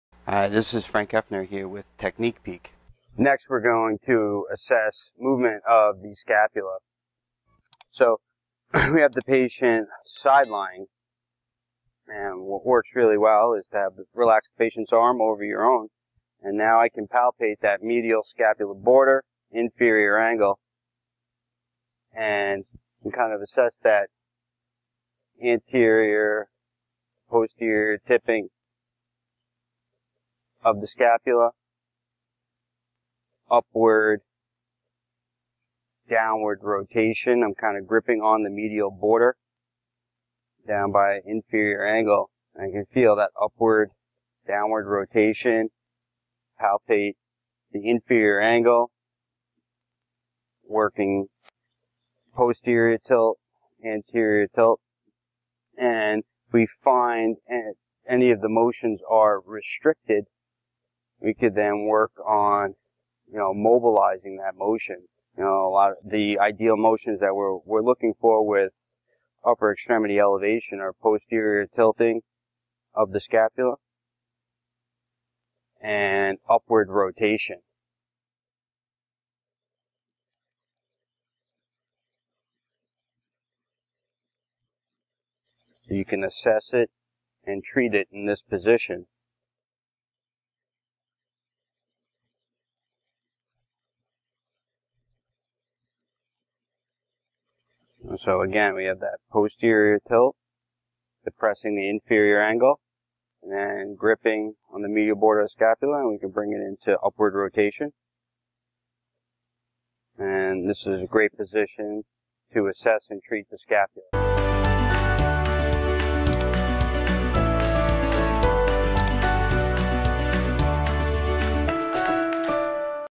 How to Assess and Mobilize the Scapula | Technique Peek Series
This Technique Peek features Frank Hoeffner, DPT, OCS, demonstrating how to Assess and mobilize the scapula when working with shoulder patients. A restricted scapula plays a significant role in decreasing a persons proper shoulder mechanics.